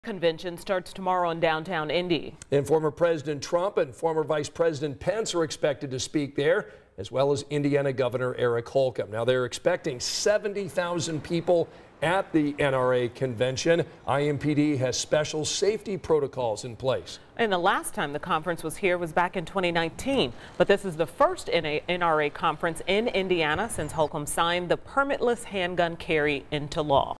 NRA convention in Indianapolis this weekend
70,000 are expected at the convention in downtown Indianapolis this weekend.